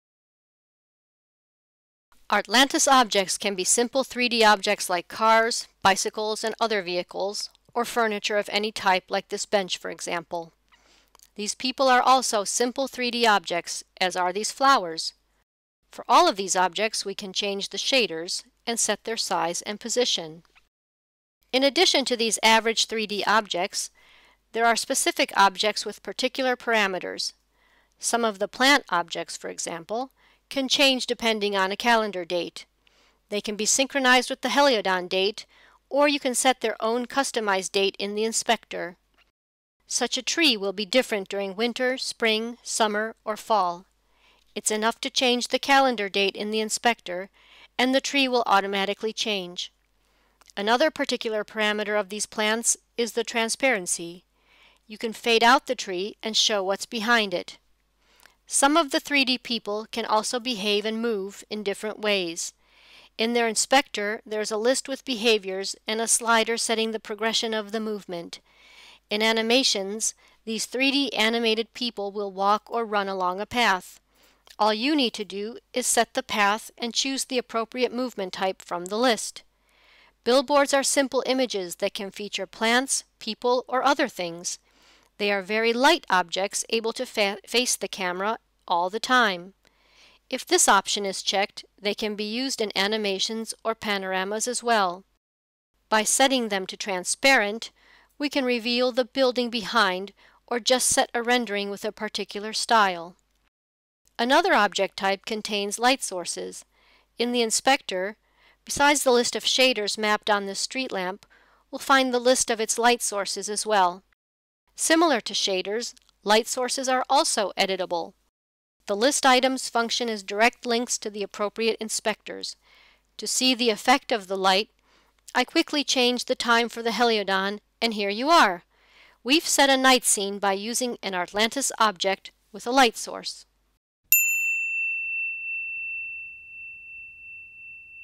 Object types in Artlantis: plants, 3D people, lamps, billboards
Artlantis objects can be simple 3D objects like cars, bicycles and other vehicles, or furniture of any type like this bench for example. These people are also simple 3D objects as are these flowers. For all these objects we can change the shaders, and set their size and position.

In addition to these average 3D objects, there are specific objects with particular parameters. Some of the plant objects, for example, can change depending on a calendar date. They can be synchronized with the Heliodon date or you can set their own customized date in the inspector. Such a tree will be different during winter, spring, summer or fall. It's enough to change the calendar date in the inspector and the tree will automatically change. Another particular parameter of these plants is the transparency. You can fade out the tree and show what's behind it.

Some of the 3D people can also behave and move in different ways. In their inspector there is a list with behaviors and a slider setting the progression of the movement. In animations, these 3D animated people will walk or run along a path. All you need to do is to set the path and choose the appropriate movement type from the list.

Billboards are simple images that can feature plants, people or other things. They are very light objects able to face the camera all the time. If this option is checked, they can be used in animations or panoramas too. By setting them transparent, we can reveal the building behind, or just set a rendering with a particular style.

Another object type contains light sources. In the inspector, besides the list of shaders mapped on this street lamp we'll find the list of its light sources too. Similar to shaders, light sources are also editable. The list items function as direct links to the appropriate inspectors. To see the effect of the light, I quickly change the time for the Heliodon and here you are! We've set a night scene by using an Artlantis object with light source.